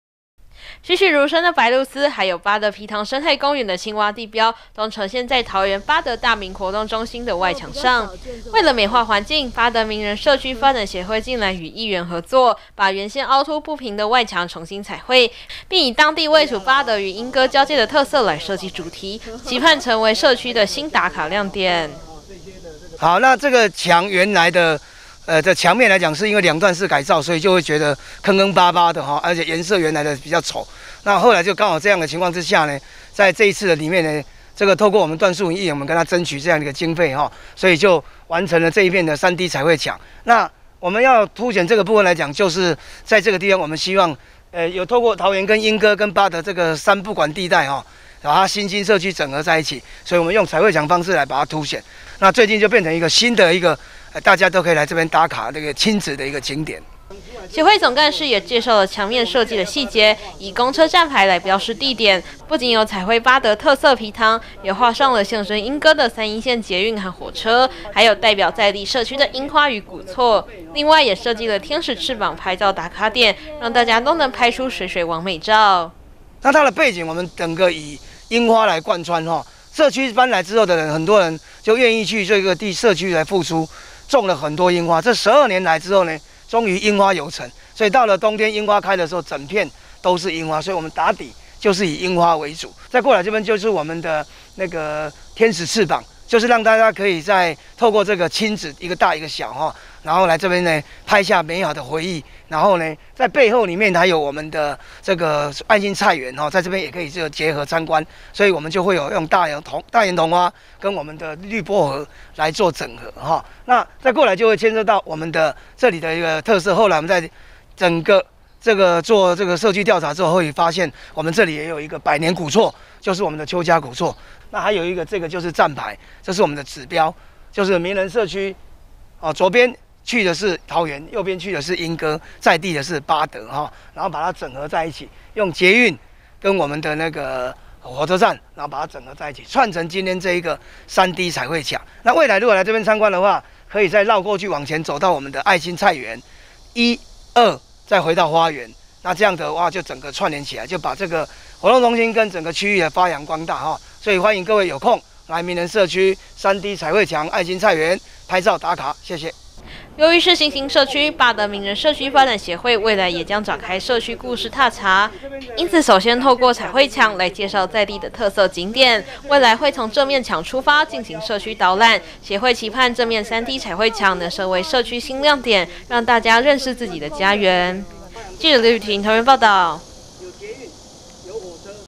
打卡 拍照|八德明仁巨幅彩繪牆 一覽社區特色記憶
八德明仁社區發展協會近來新增設彩繪牆成打卡拍照新亮點

[中嘉新聞/北健/桃園八德報導]
桃園八德明仁社區發展協會近來向議員申請補助，在活動中心外牆彩繪出當地位處八德與鶯歌交界的特色，不僅有八德特色埤塘，也有象徵鶯歌的捷運與火車，還有社區的櫻花與古厝，更設計拍照打卡點，協會期盼3D彩繪牆未來能成為社區新亮點。